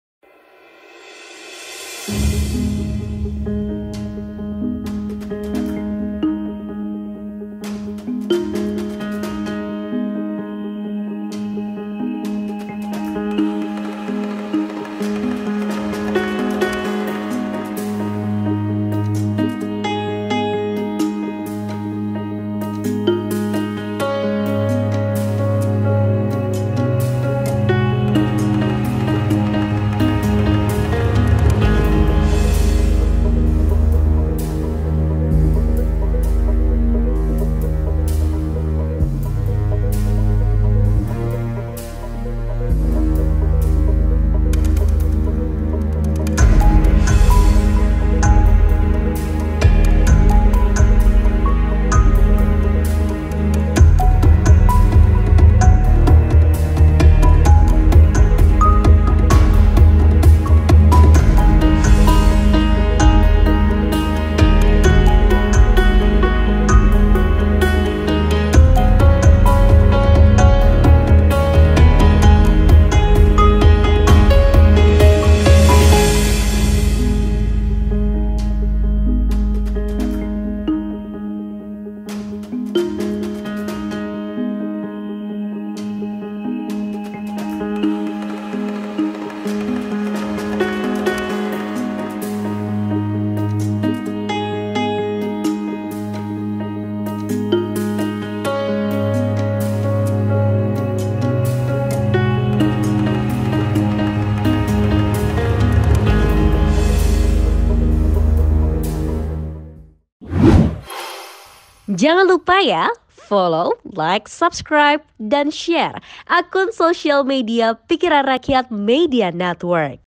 Wakil Ketua DPRD Jatim Terjaring OTT, KPK Ungkap Penyebabnya
Komisi Pemberantasan Korupsi (KPK) melakukan operasi tangkap tangan (OTT) terhadap sejumlah pihak di Surabaya, pada Rabu malam, 14 Desember 2022.

Dalam keterangannya, Ketua Komisi Pemberantasan Korupsi (KPK), Firli Bahuri menyebut salah satu pihak yang terjaring OTT berinisial STS, pengemban jabatan Wakil Ketua DPRD Jawa Timur.

Ada pun STS turut diamankan KPK atas kasus dugaan maling uang rakyat dalam proyek alokasi dana hibah.

"KPK ungkap dugaan korupsi dana hibah ke kelompok masyarakat dalam giat tangkap tangan Wakil Ketua DPRD Jatim STS dan pihak lain," yjar Firli.